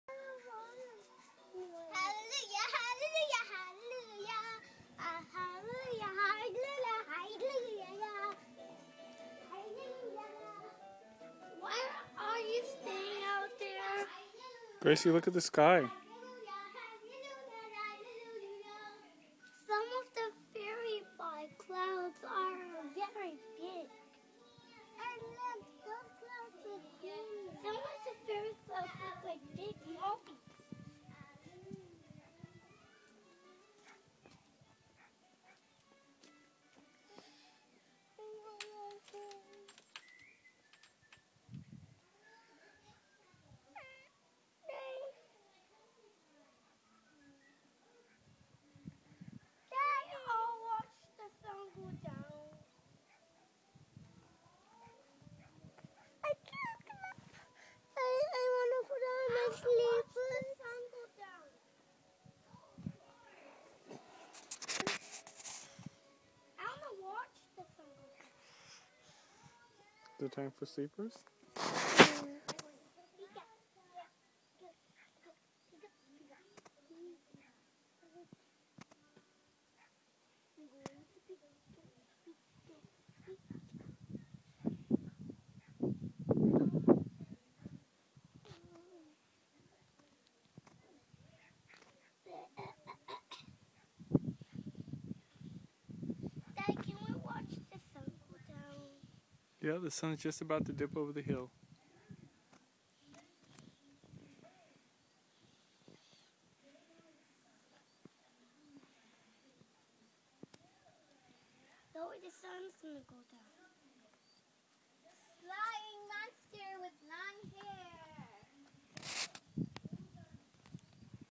Our Beautiful Sky
This is one of those rare occasions when I take a video, then upload it immediately. Why such whispy clouds, that turn like they do? Absolutely beautiful.

IMG_0388.MOV